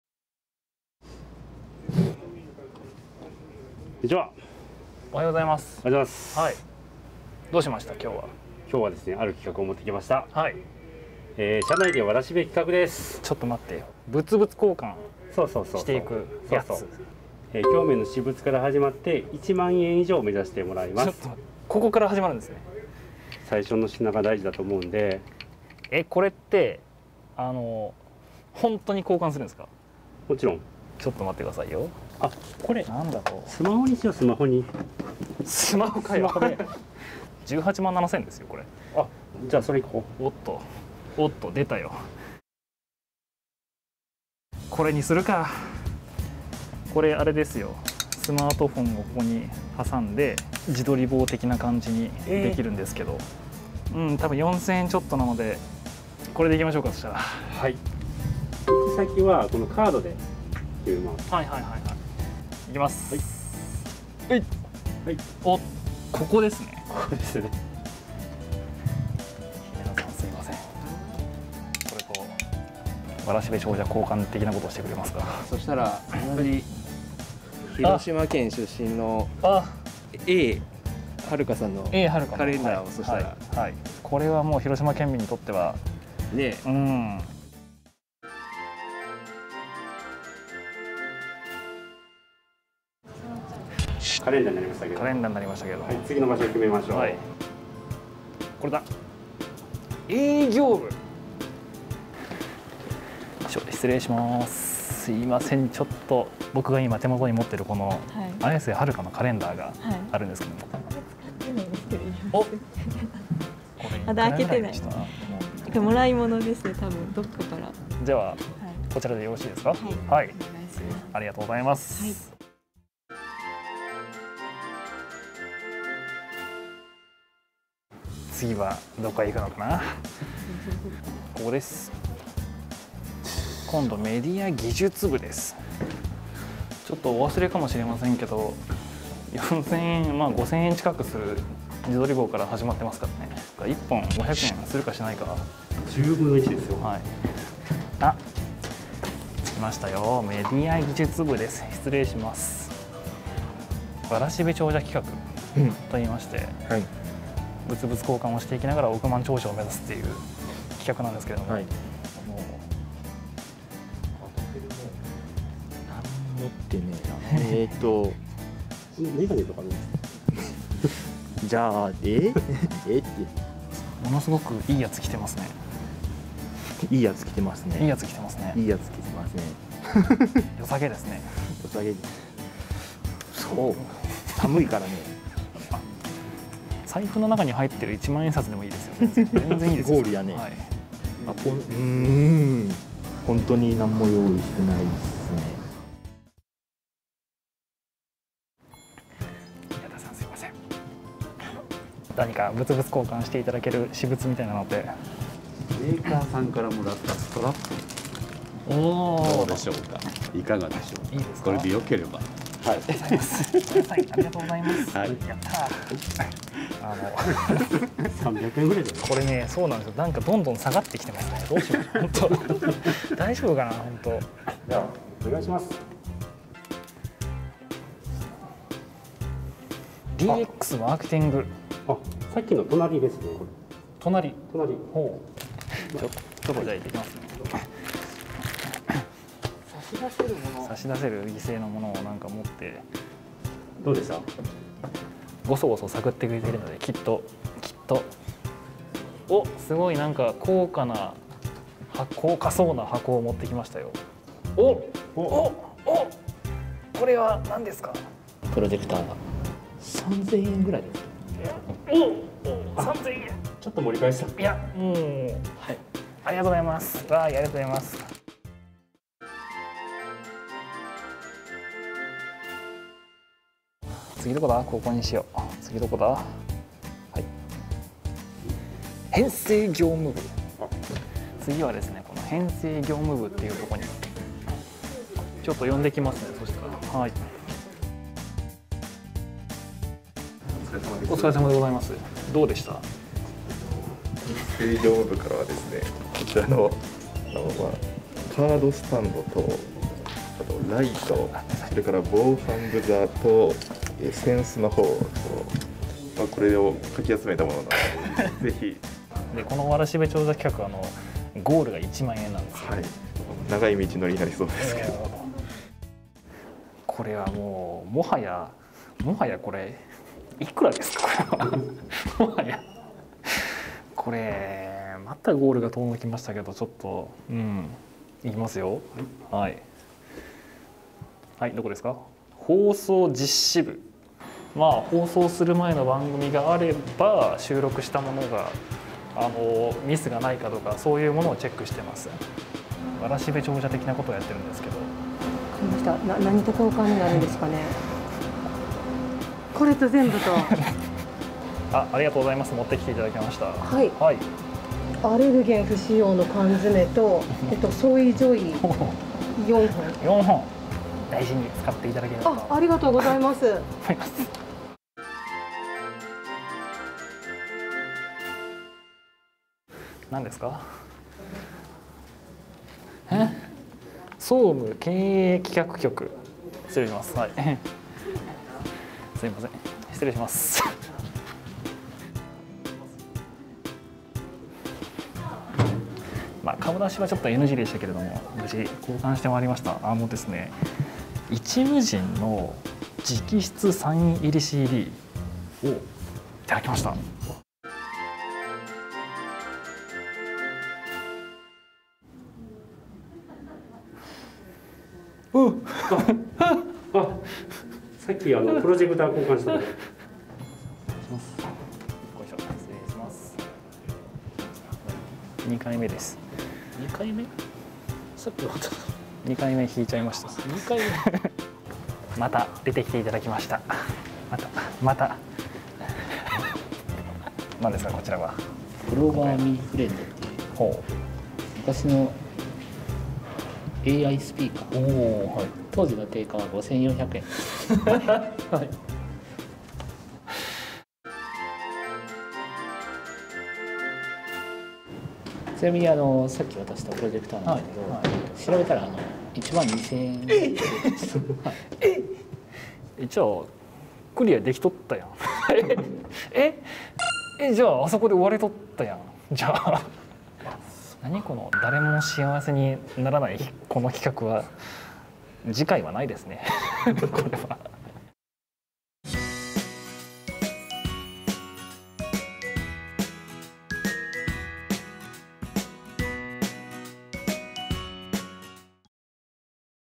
目指せ１万円！テレビ高知的わらしべ長者
昔話の「わらしべ長者」のように、社内を巡りながら物々交換を続け、１万円以上の品物を目指します。京面アナウンサーの私物が次々と意外な品物に変わっていき・・・ラストには衝撃の結末が！果たしてゴールにたどり着けるのか？！

■テレビ高知公式YouTubeでは普段テレビでは見ることのできないアナウンサーの素顔や裏側、オリジナルコンテンツを投稿しています！